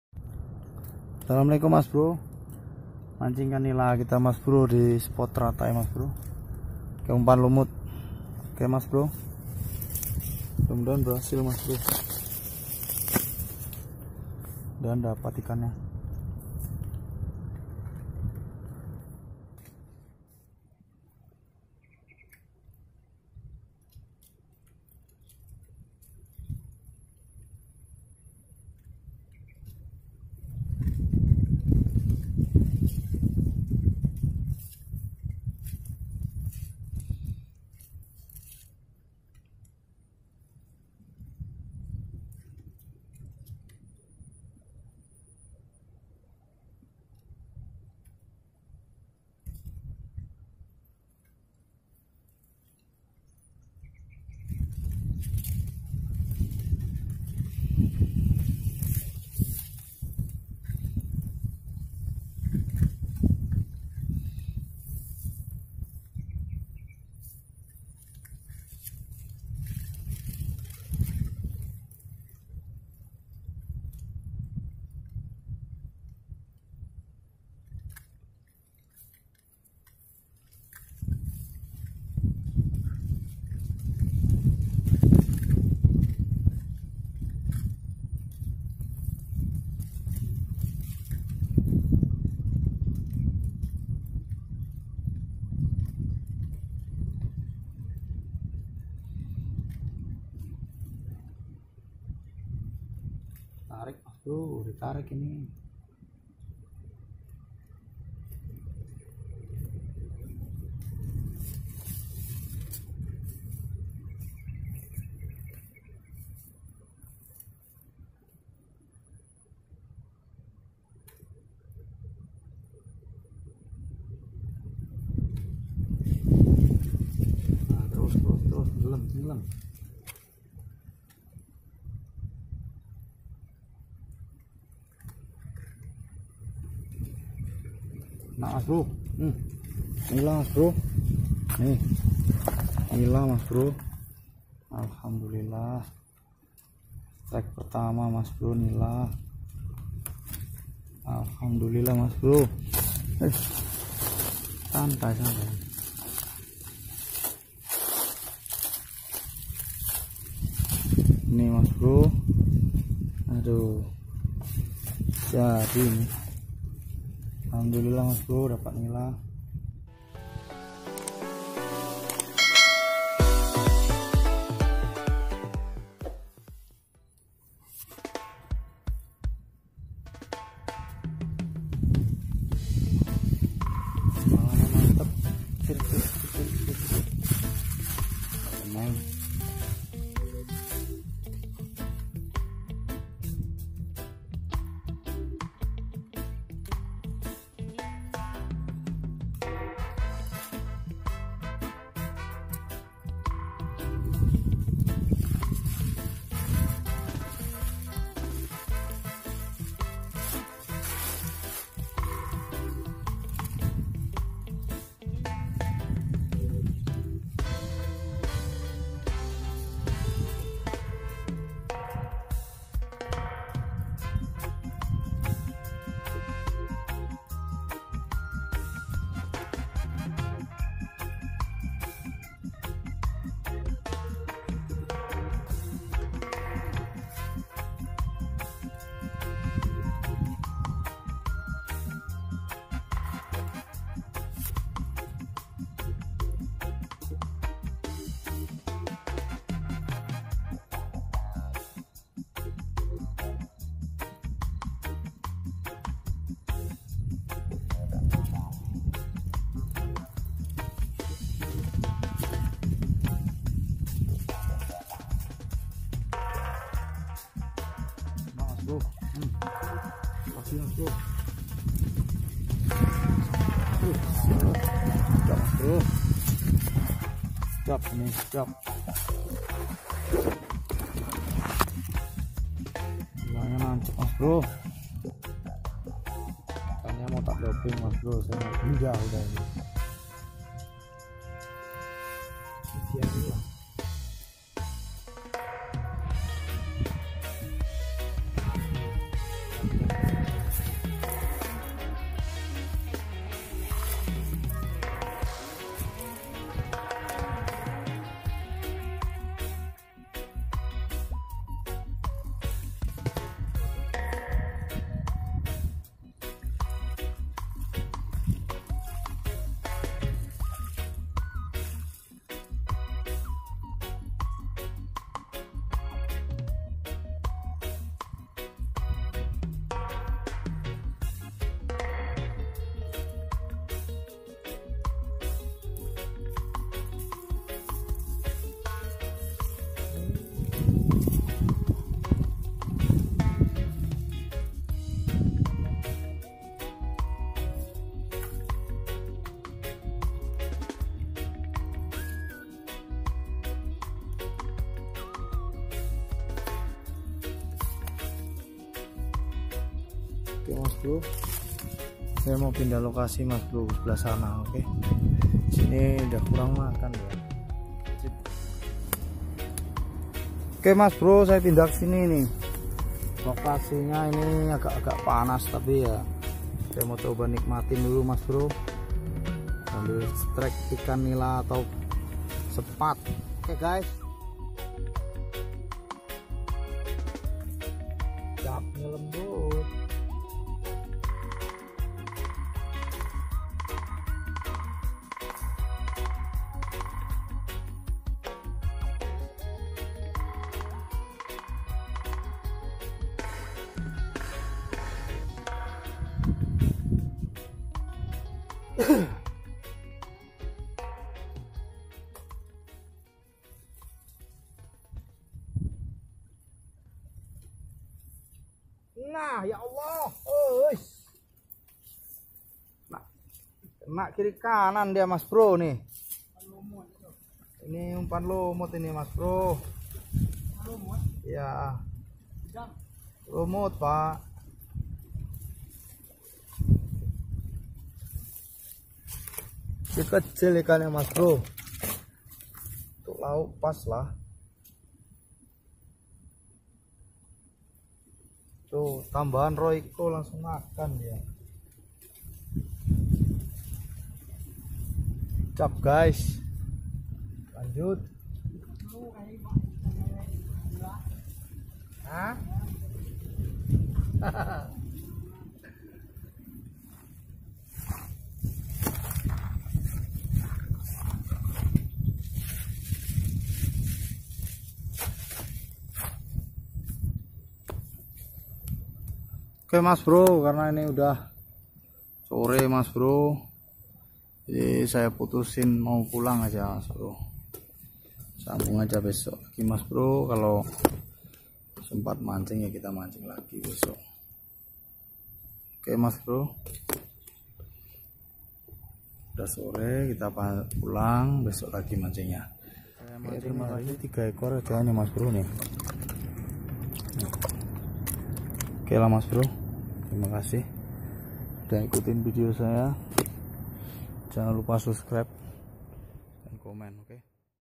Mancing nila spot teratai
Asalamualaikum mas bro kali ini masih mancing nila di sepot teratai. Hasil nya alhamdulilah.